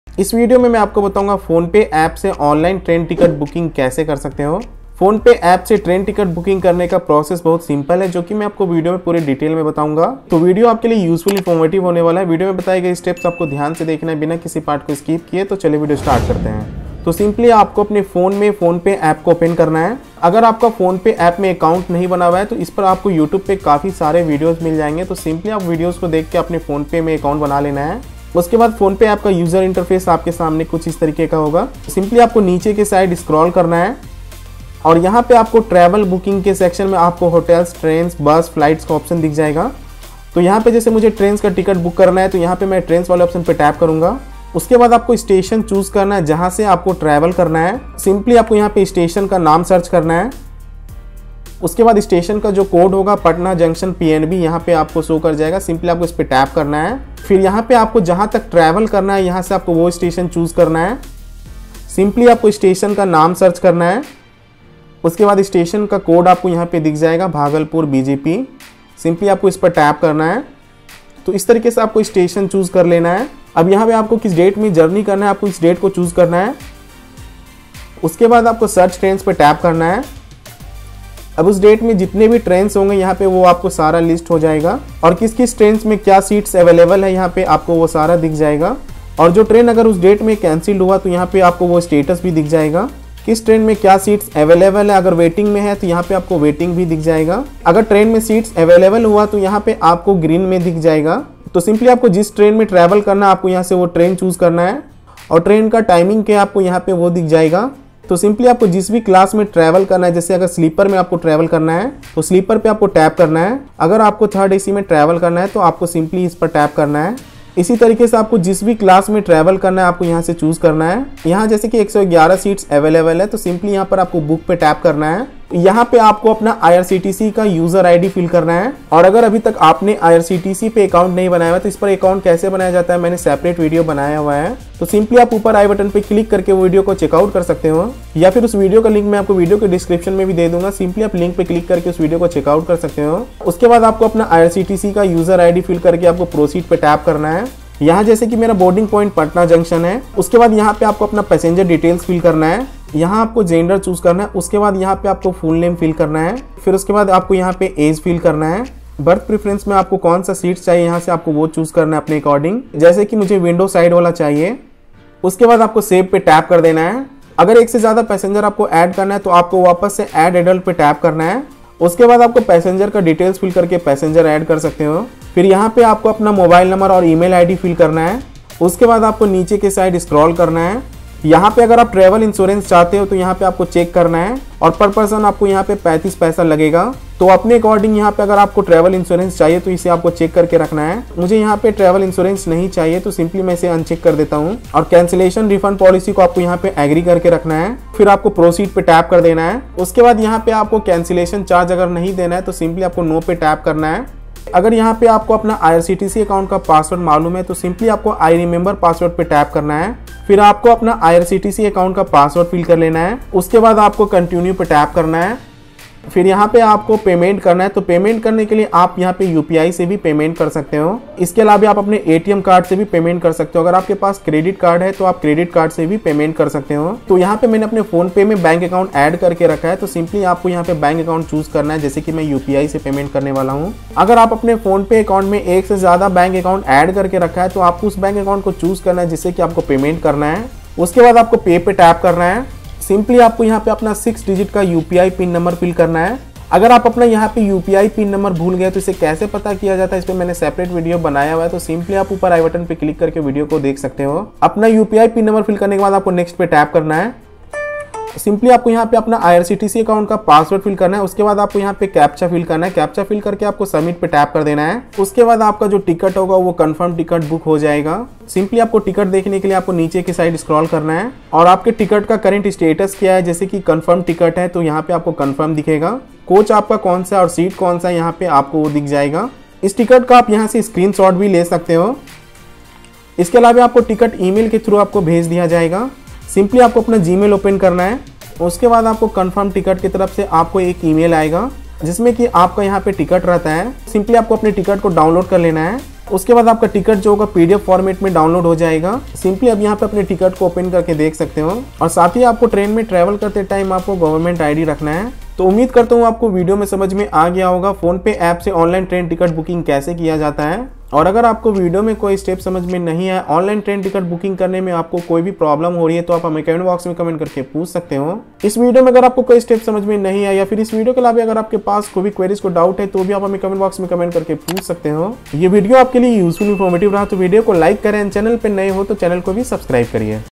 train ticket booking online | phonepe se train ticket kaise book kare
es video me maine bataya hain phonepe app se online train ticket confirm booking kaise kare.